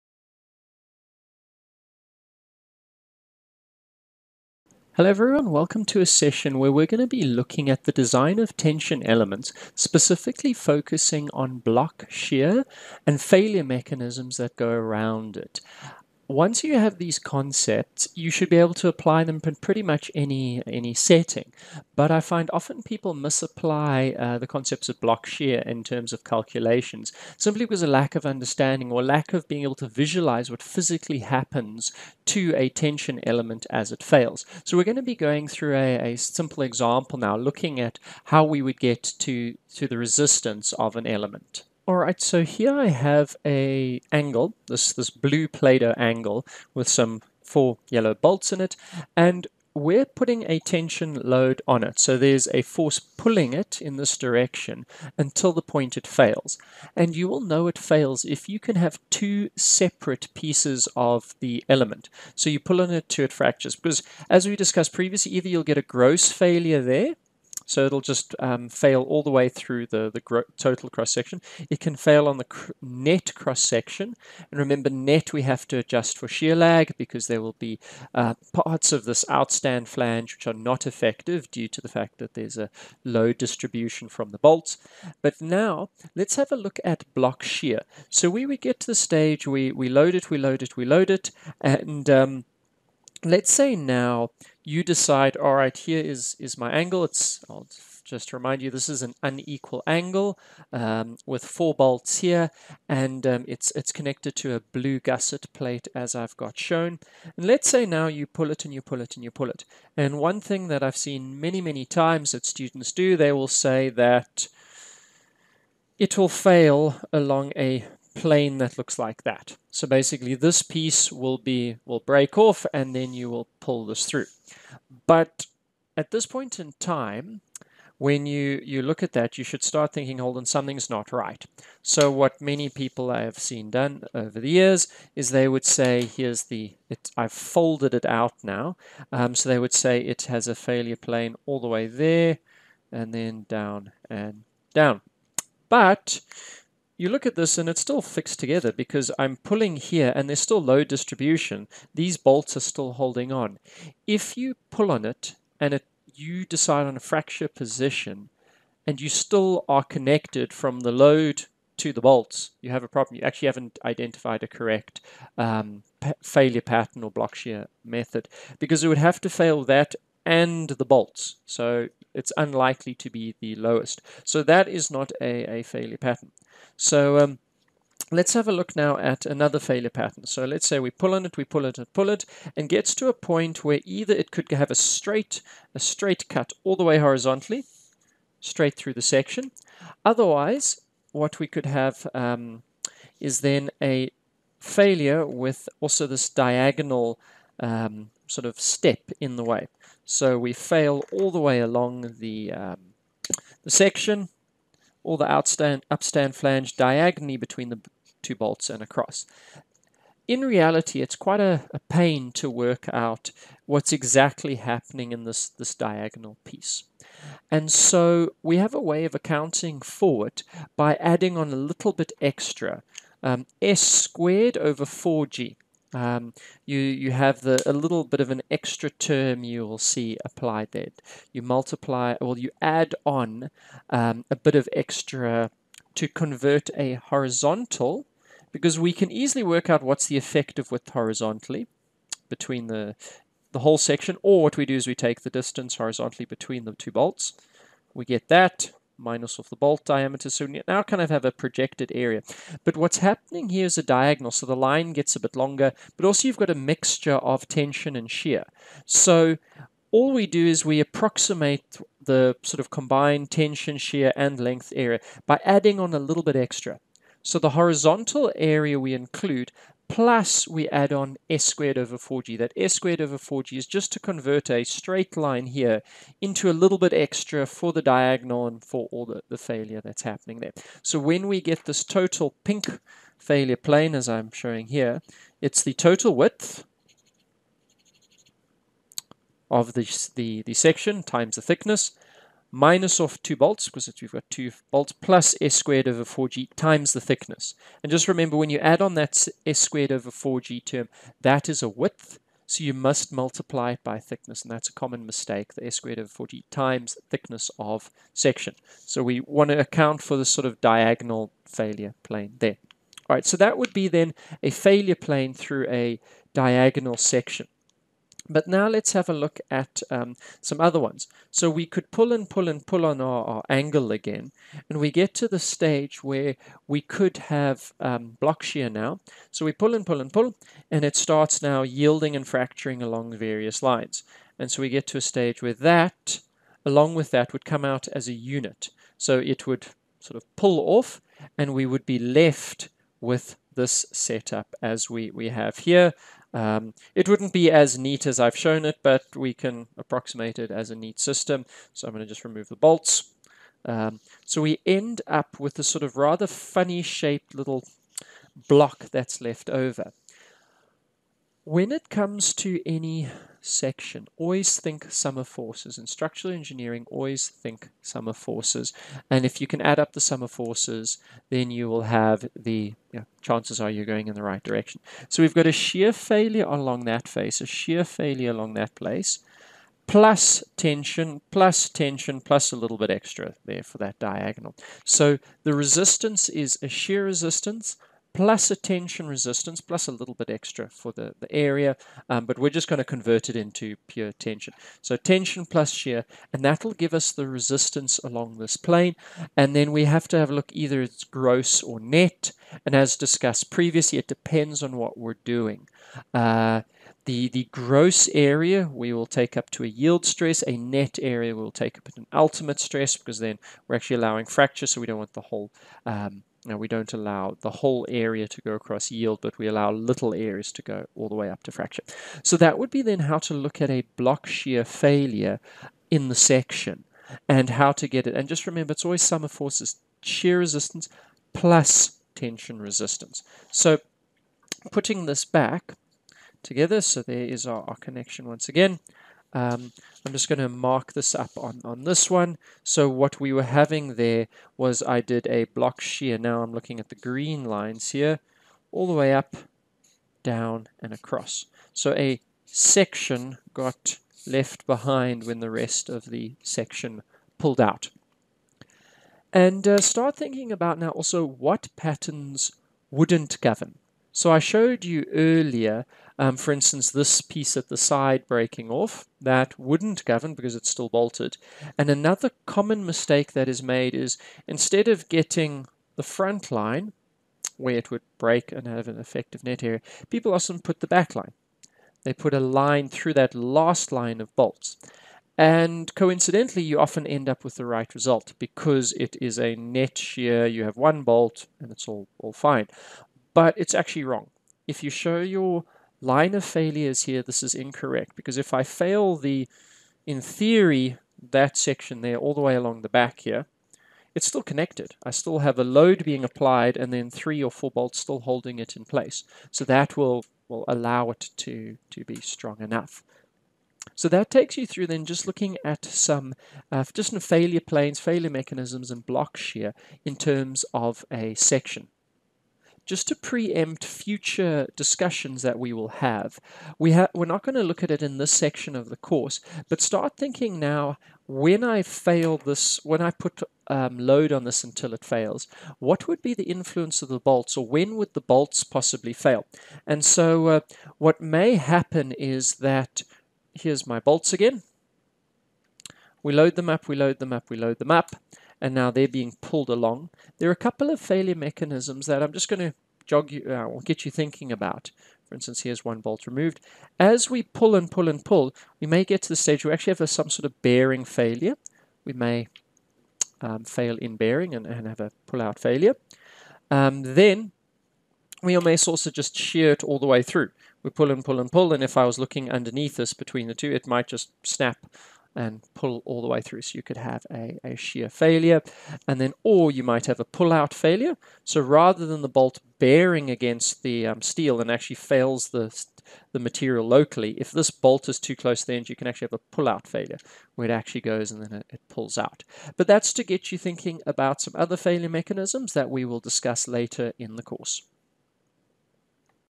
Block shear and failure mechanisms - Steel members in tension - SD424
Here we play with some playdough to try explain failure mechanisms and block shear in tension elements.

Produced for the SD424 Structural Design course.
Copyright Stellenbosch University.